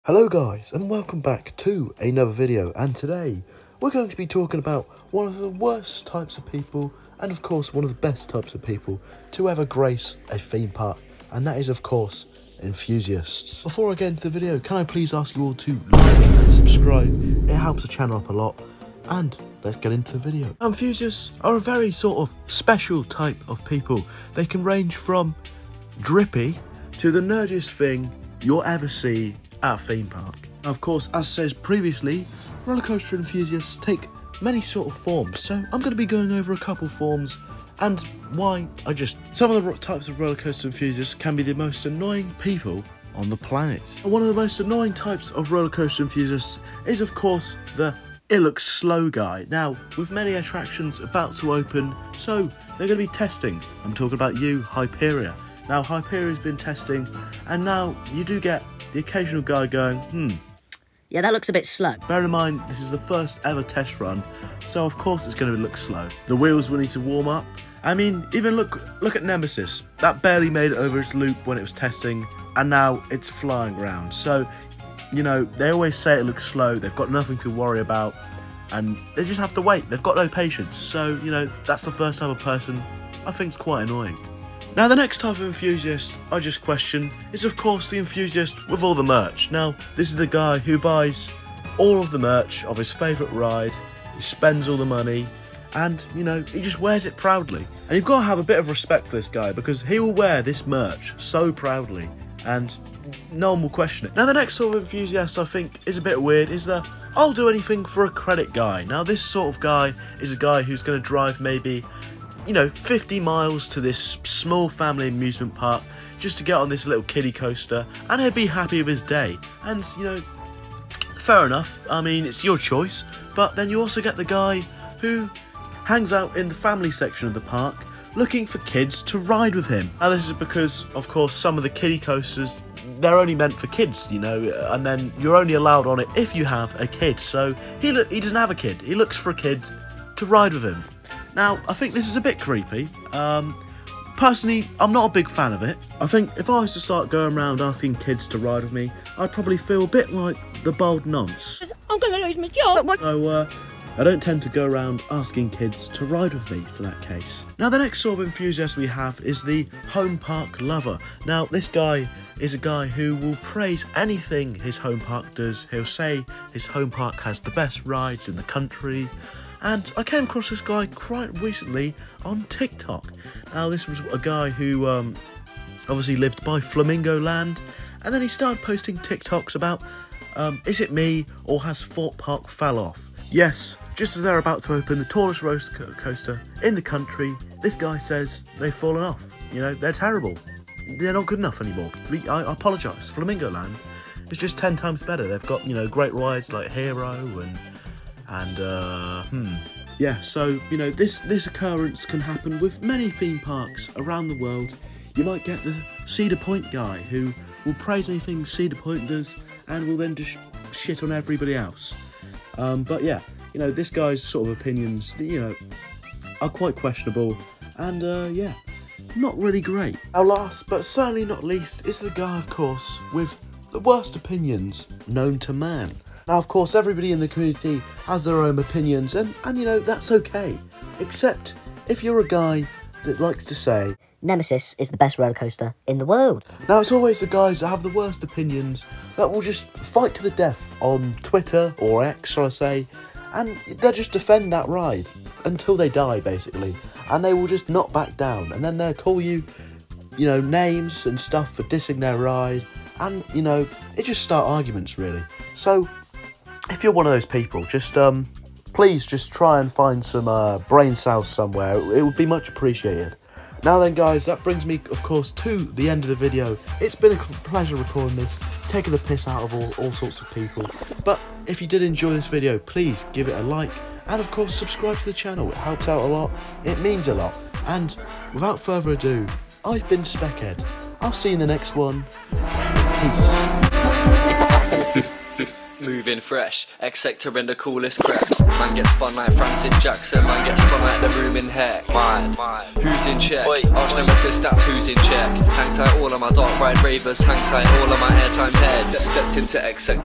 The Worst Types of Rollercoaster Enthusiasts
In this video, we're diving into the worst types of rollercoaster enthusiasts that you might encounter.

All credit for footage goes too coaster bot and jack silkstone